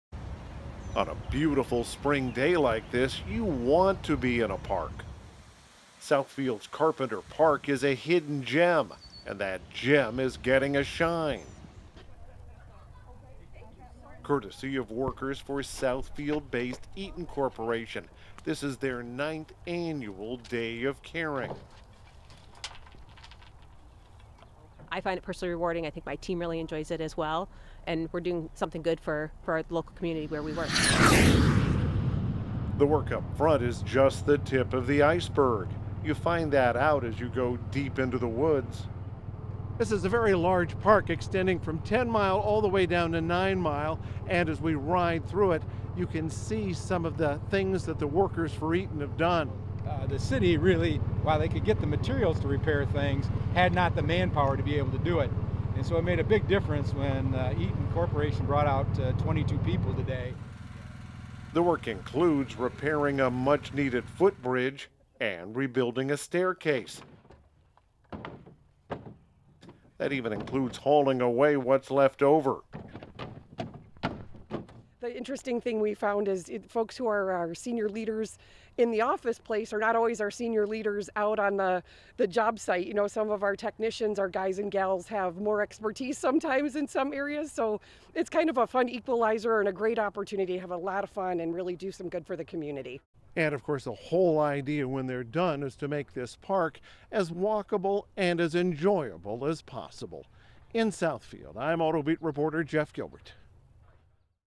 Auto Supplier Eaton Corporation Cleans Up Local Parks
Workers for Southfield based auto supplier Eaton Corporation spend a couple of days cleaning up two parks.  WWJ AutoBeat Reporter Jeff Gilbert talks with some of them.